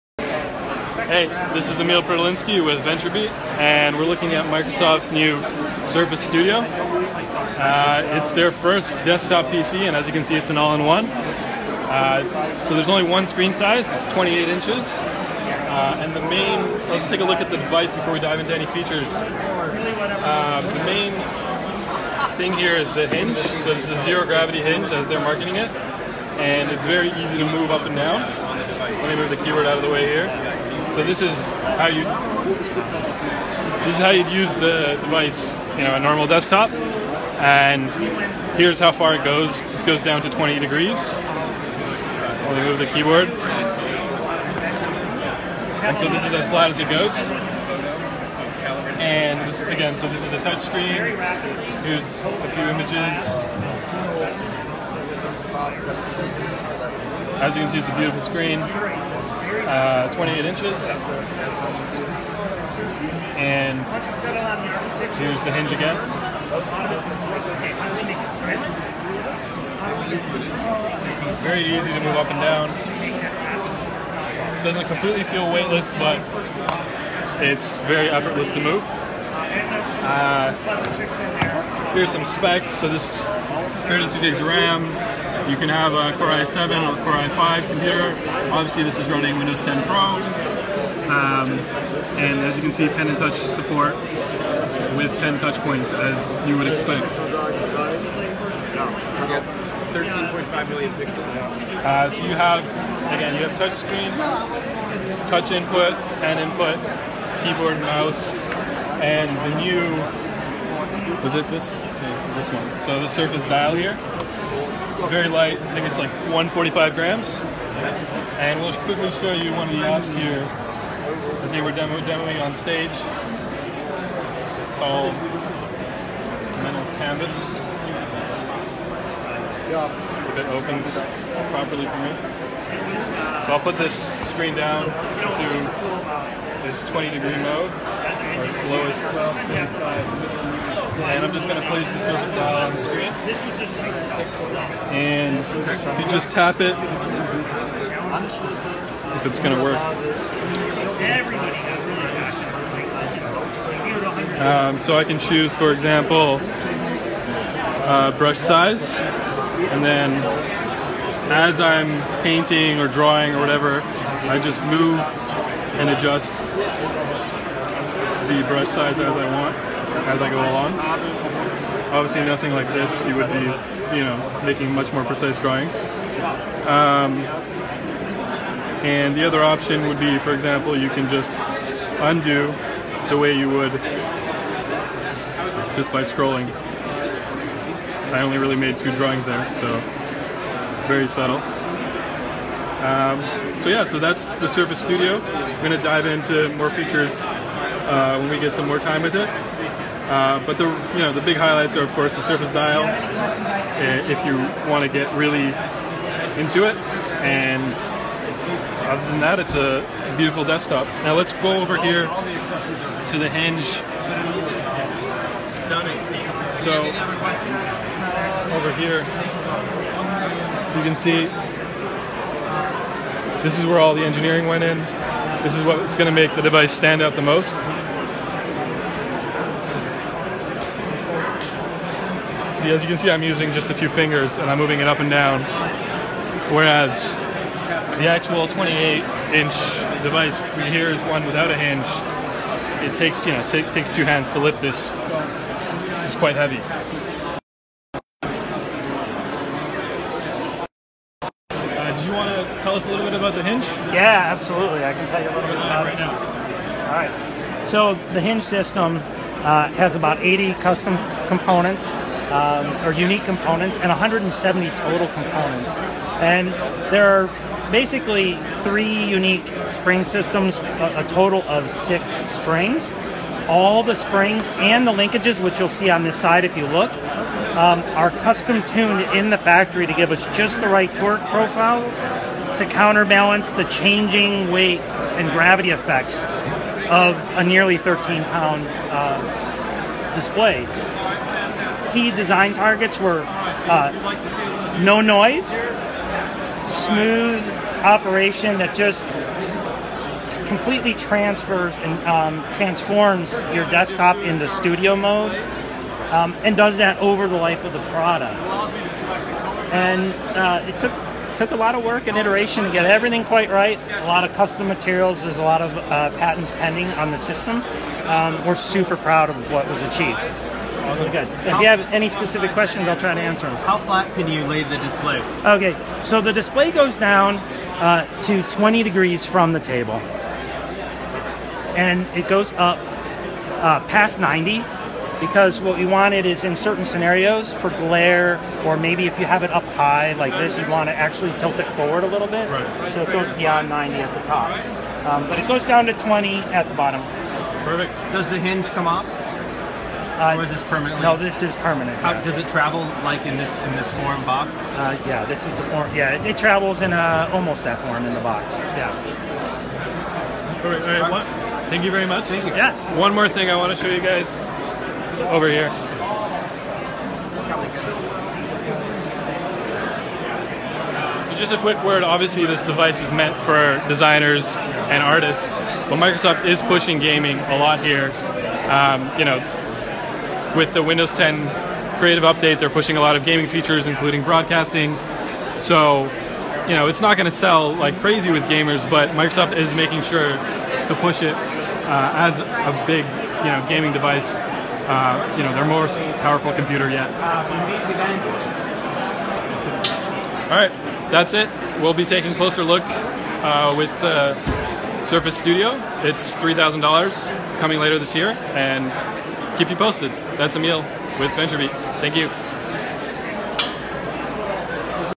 Microsoft's Surface Studio and the Surface Dial launch Event - Review
Microsoft's Surface Studio and the Surface Dial comes with:
i5/i7 Intel® Core™ Processor

Up to 4 GB NVIDIA® GeForce® GPU

Up to 32 GB RAM